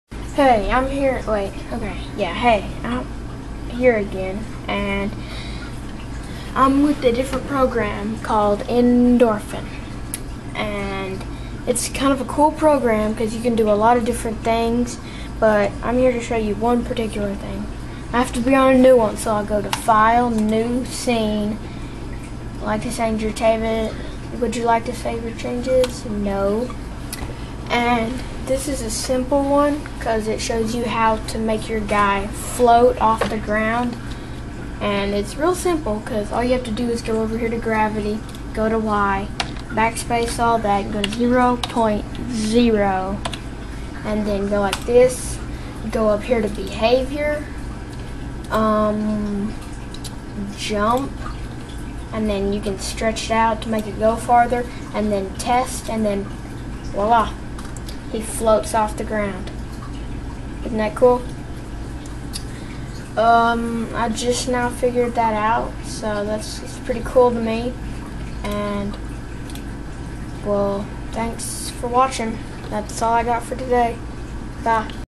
Endorphin - Floating character
I found out how to make my guy float in Endorphin.